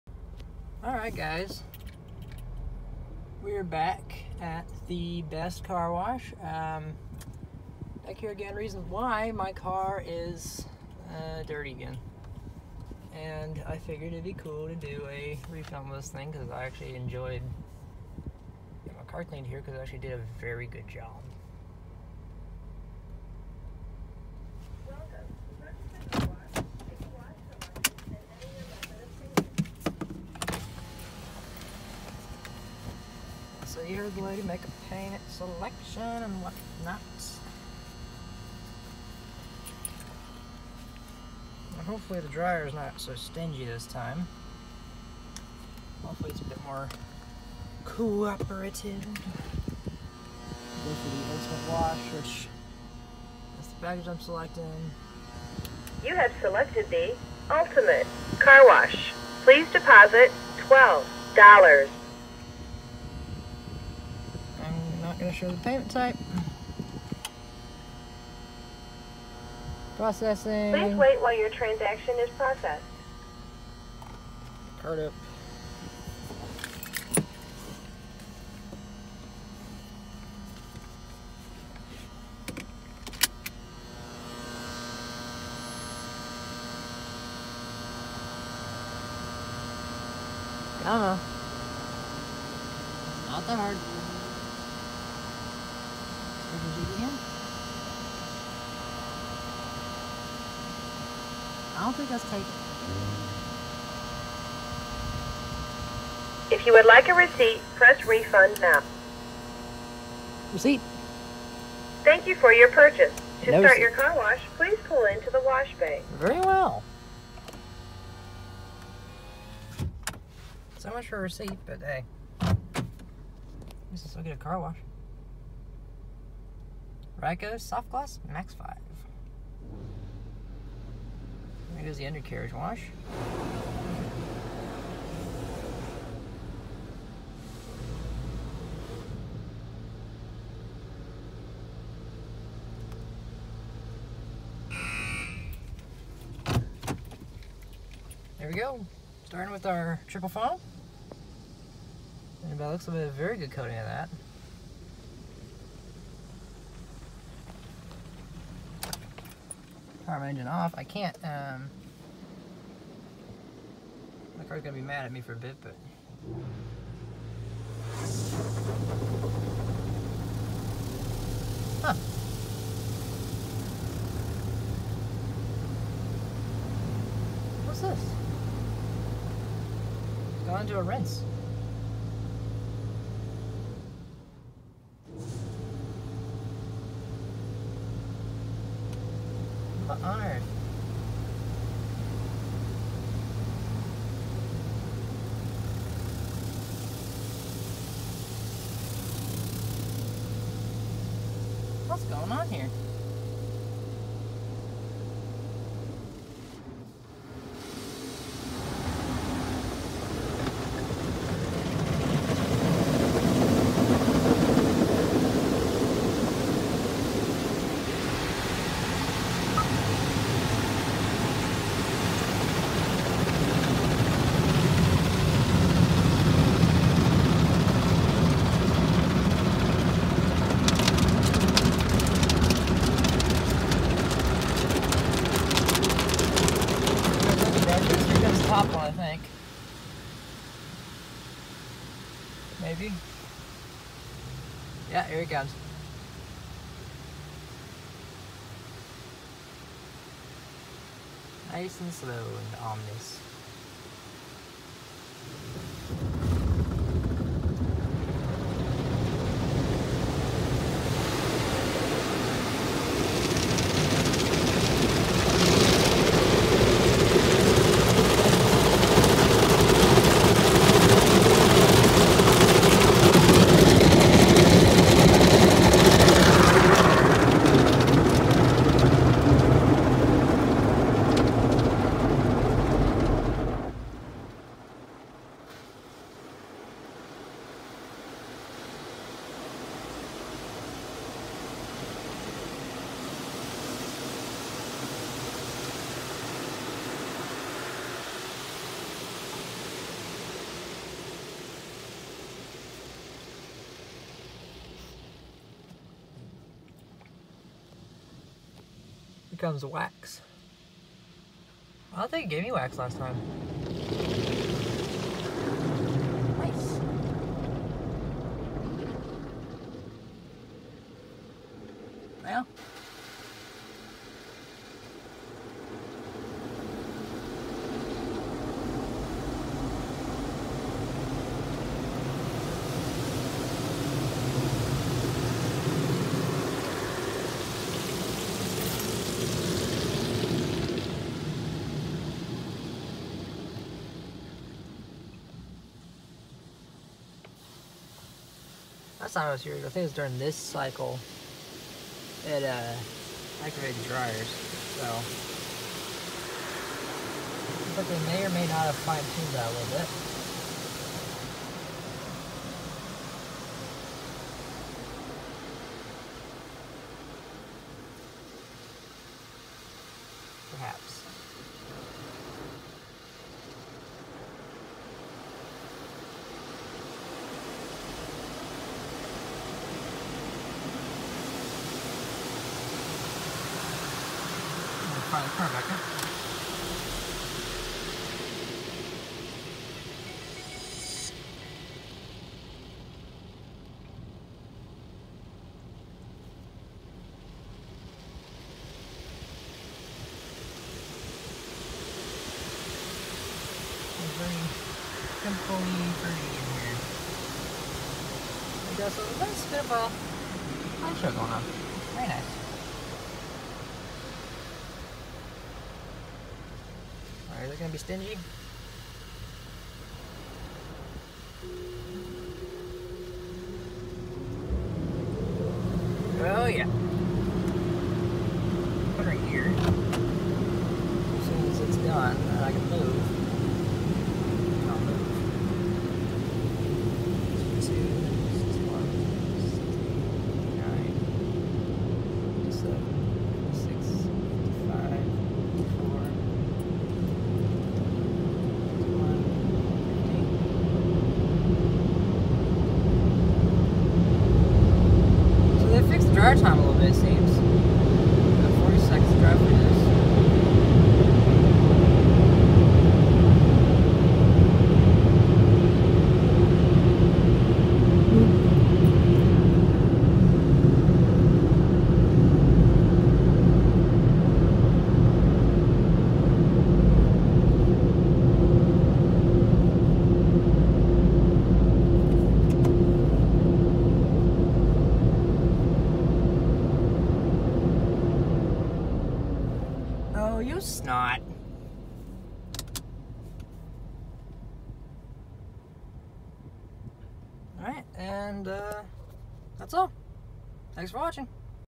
An update on the Ryko Softgloss Maxx 5 @ The Perfect Car Wash, Tulsa, OK
After about two weeks of clean my car was dirty once more. As I was on that side of town I figured I’d pay this wash a visit and I’ve noticed a few slight improvements. Enjoy!

Manufacturer: Ryko
Year: ?
Model: Softgloss Maxx 5
Type: Car Wash
Dryers: Ryko Thrustpro
Wash Package: Ultimate ($12)
Package details: N/A (already shown in video)
Rating: 10/10
The Wash overall did a wonderful job of cleaning my car and it seems like the dryers are a little less stingy.